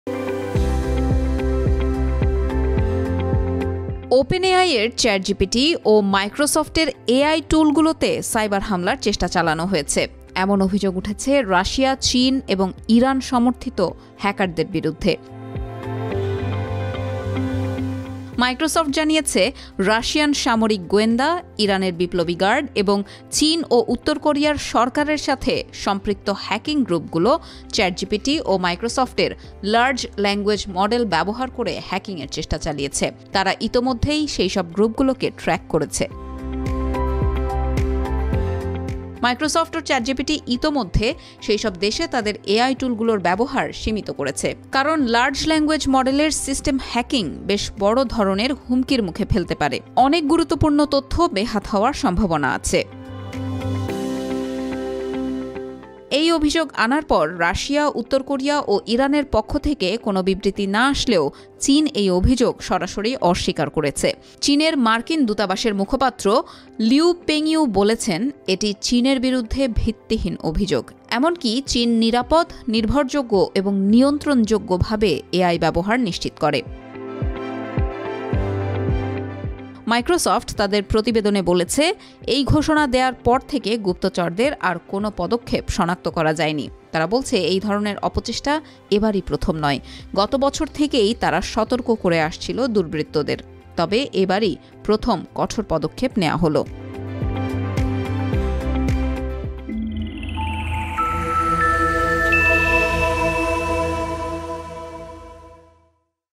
চীন-রাশিয়া-ইরানের বিরু'দ্ধে সাই'বার হা'মলা চেষ্টার অভি'যোগ চ্যাটজিপিটি ও মাইক্রোসফটের
Microsoft says it caught hackers from China, Russia and Iran using its AI tools
ওপেন এআই এর চ্যাটজিপিটি ও মাইক্রোসফট এর এ আই টুলগুলো হ্যাক করার চেষ্টা করছে রাশিয়া, চীন এবং ইরানের রাষ্ট্র-সমর্থিত হ্যাকাররা। এমনটাই বলছে সিএনবিসির একটি প্রতিবেদন। মাইক্রোসফট জানিয়েছে রাশিয়ান সামরিক গোয়েন্দা, ইরানের বিপ্লবী গার্ড এবং চীন ও উত্তর কোরিয়ার সরকারের সাথে সম্পৃক্ত হ্যাকিং গ্রুপগুলো চ্যাট জিপিটি ও মাইক্রোসফটের লার্জ ল্যাঙ্গুয়েজ মডেল ব্যবহার করে হ্যাকিং এর চেষ্টা চালিয়েছে। তারা ইতোমধ্যেই সেইসব গ্রুপগুলোকে ট্র্যাক করেছে।